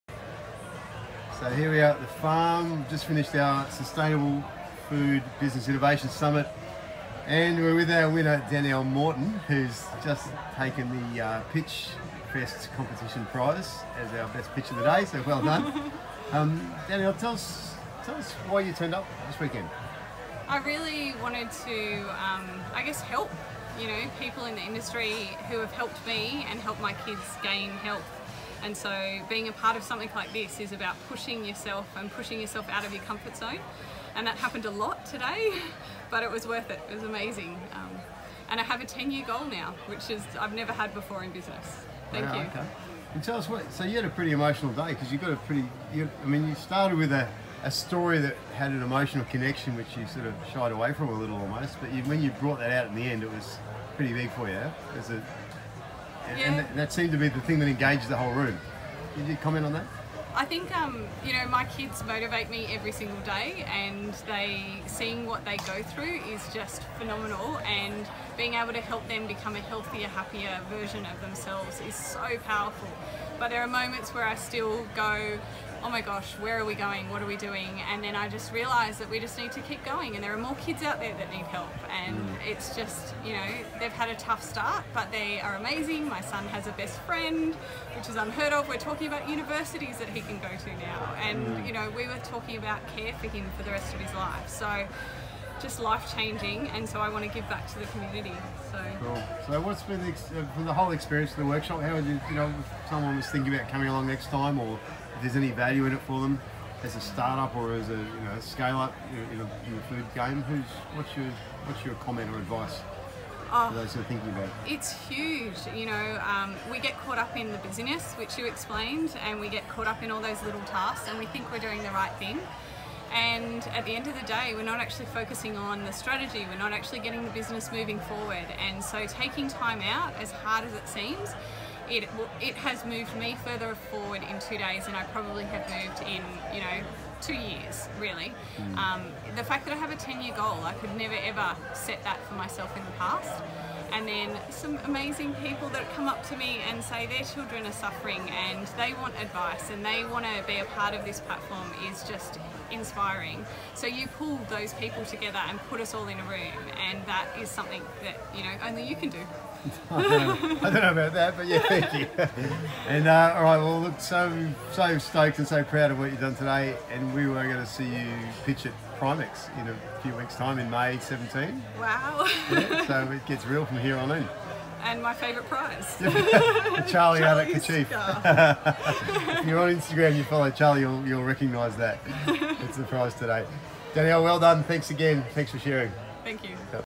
"It's HUGE! I've moved further forward in two days than I have in the last two years!" - Danielle
Danielle Morton interviewed after winning the first Venture Organic Pitch Event